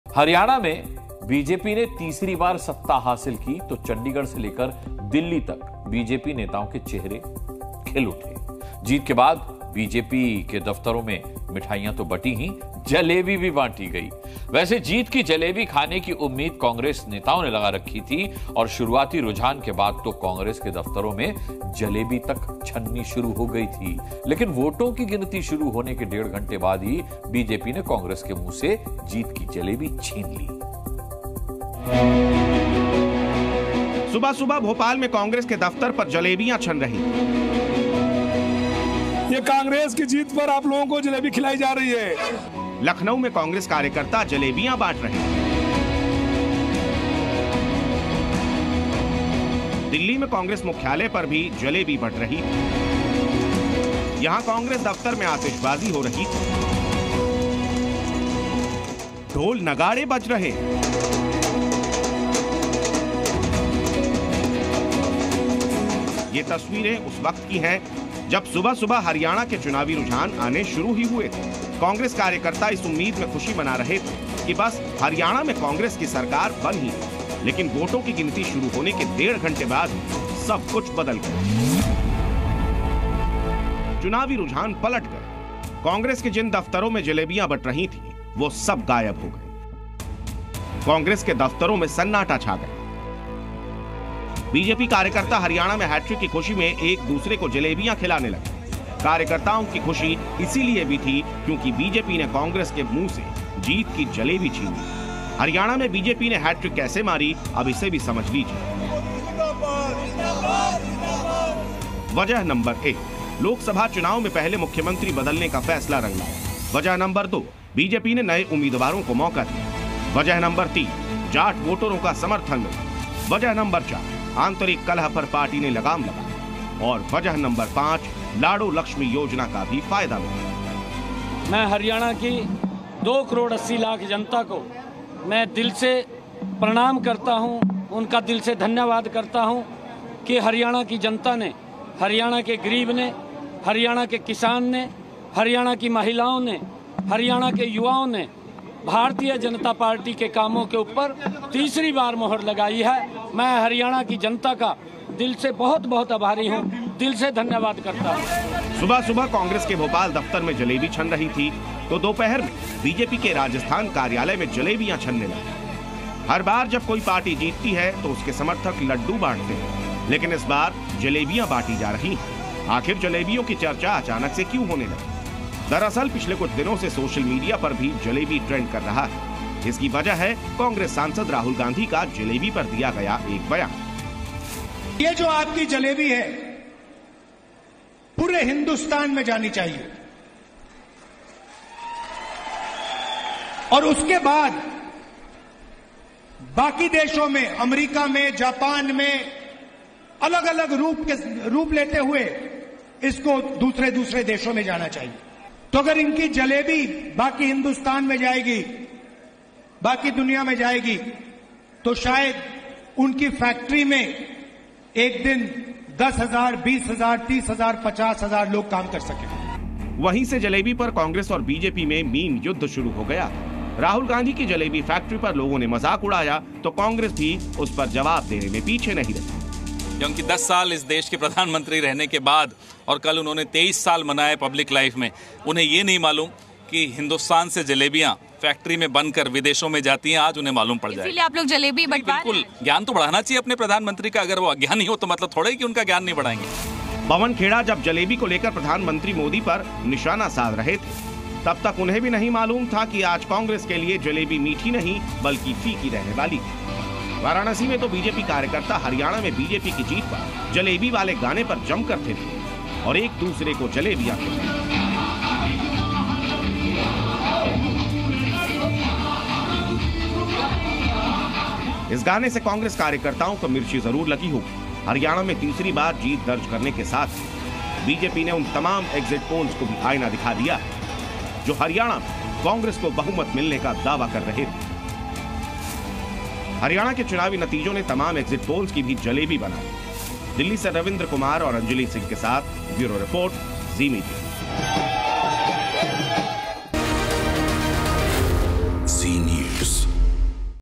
BJP Wins Haryana Election Update: कांग्रेस से कैसे BJP ने छीनी जलेबी?Celebration | Result 2024 |Modi
Haryana Jammu Kashmir Election Results Update: हरियाणा में बीजेपी ने तीसरी बार सत्ता हासिल की तो चंडीगढ़ से लेकर दिल्ली तक बीजेपी नेताओं के चेहरे खिल गए । जीत के बाद बीजेपी के दफ्तरों में मिठाईयां तो बंटी ही, जलेबी भी बांटी गई । वैसे जीत की जलेबी खाने की उम्मीद कांग्रेस नेताओं ने लगा रखी थी...और शुरुआती रुझान के बाद तो कांग्रेस के दफ्तरों में जलेबी तक छनने शुरू हो गए थे । लेकिन वोटों की गिनती शुरू होने के डेढ़ घंटे बाद ही बीजेपी ने कांग्रेस के मुंह से जीत की जलेबी छीन ली ।

Haryana Jammu Kashmir Election Results Update: When BJP won power for the third time in Haryana, the faces of BJP leaders from Chandigarh to Delhi lit up. After the victory, not only sweets were distributed in BJP offices, but jalebis were also distributed. Congress leaders were hoping to eat the jalebi of victory...and after the initial trends, even jalebis started being distributed in Congress offices. But just one and a half hours after the counting of votes began, BJP snatched the jalebi of victory from Congress's mouth.